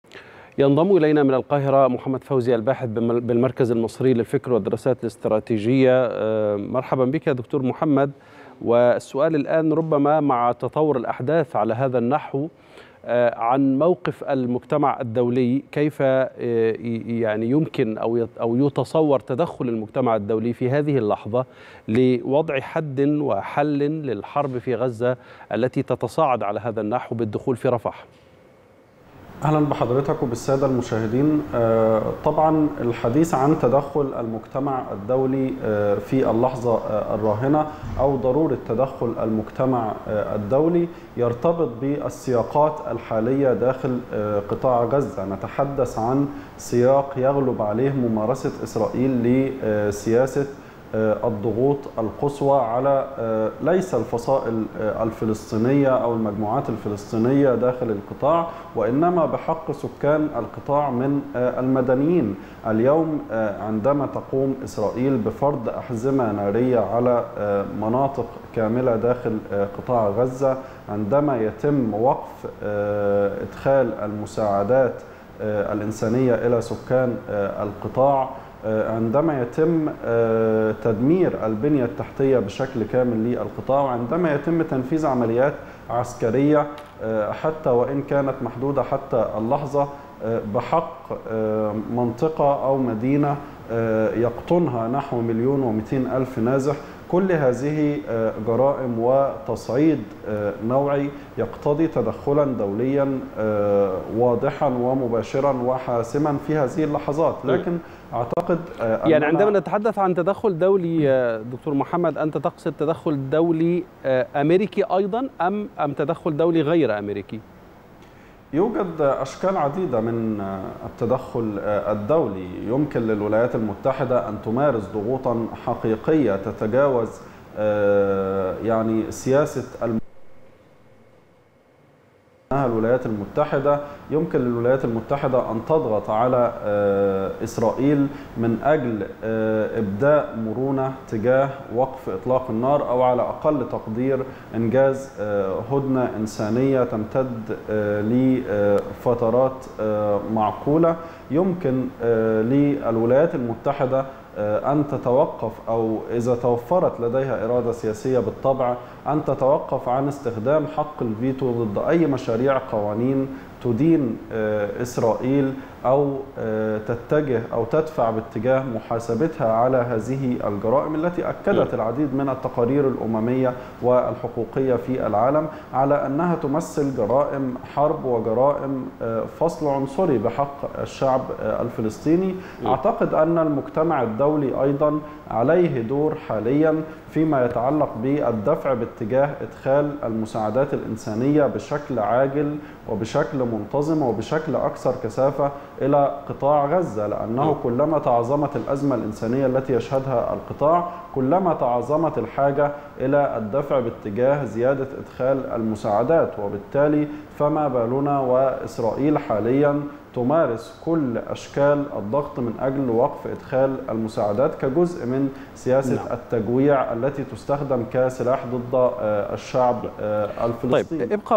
محمد فوزي الباحث بالمركز المصري للفكر: الجرائم الإسرائيلية في غزة تقتضي وجود تدخل دولي لوقفها
8/5/2024